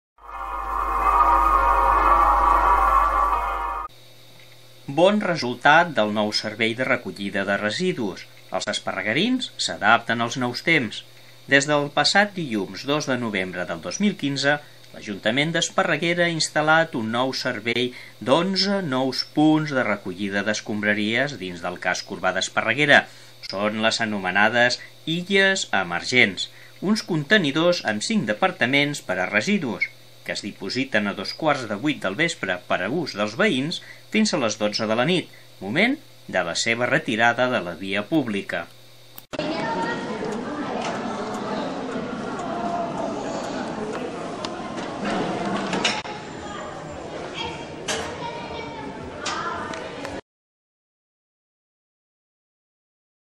ESPARREGUERA TV. Les Illes Emergens, nou servei de recollida d'escombreries a Esparreguera
Nou Servei de Recollida de residus.
Bona acollida del nou Servei de Recollida de residus al casc urbà d'Esparreguera, amb les "Illes Emergents" que només funcionen de 20:30 a 24:00 hores; de l'Ajuntament d'Esparreguera.

ESPARREGUERA TV